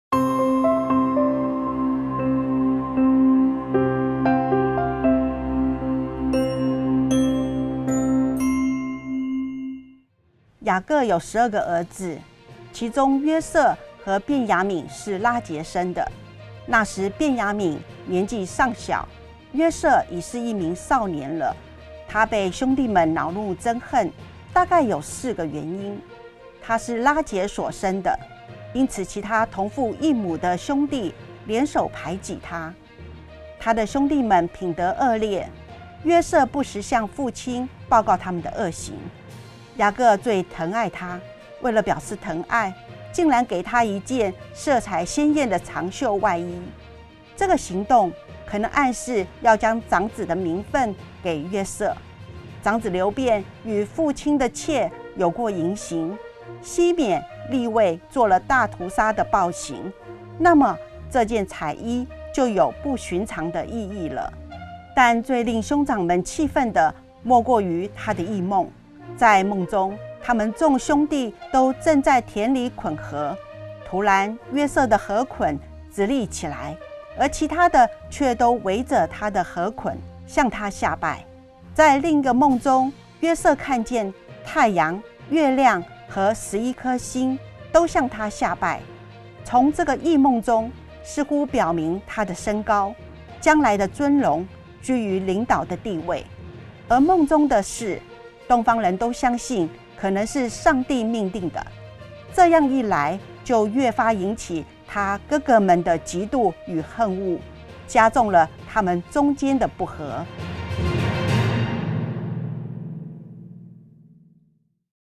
【創世記疑難排解】45. 約瑟的眾兄弟為什麼會恨他？
雅各過於疼愛約瑟，
招致他的哥哥們嫉妒與恨惡約瑟，
究竟是那些因素而引起的呢?
歡迎收看真光短視頻的解答。